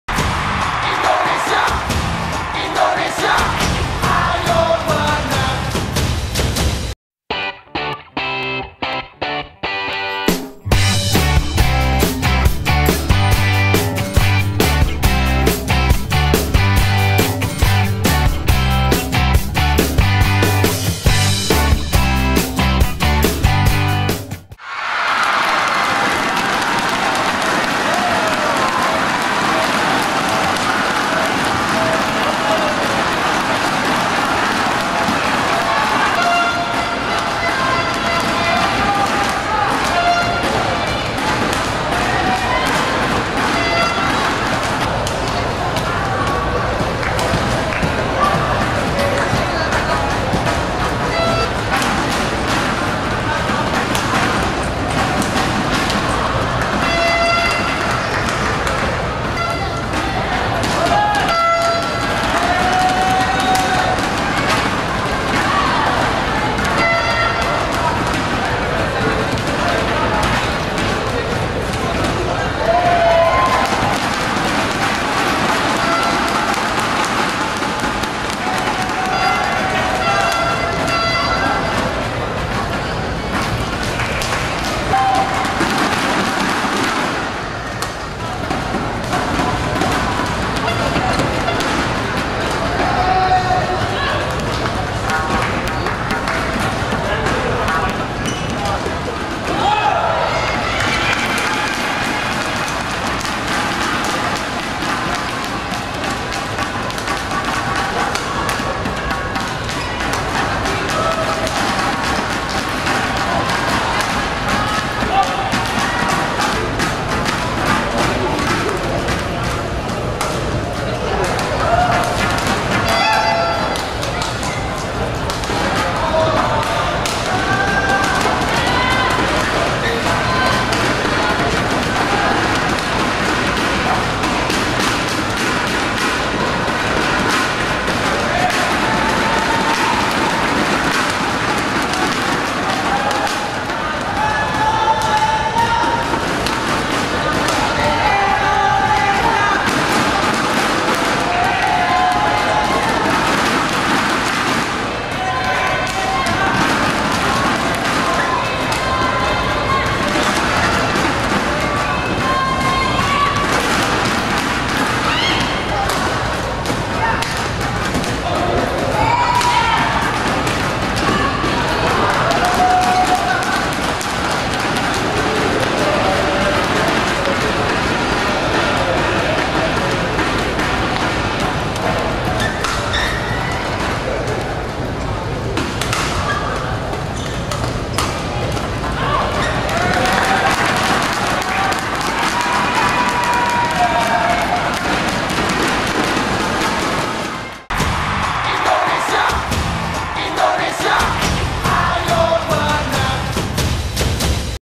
Tine Baun (DEN) VS Petya Nedelcheva (BUL) Djarum Indonesia Open 2012
Tine Baun (Denmark) VS Petya Nedelcheva (Bulgaria)  at Djarum Indonesia Open 2012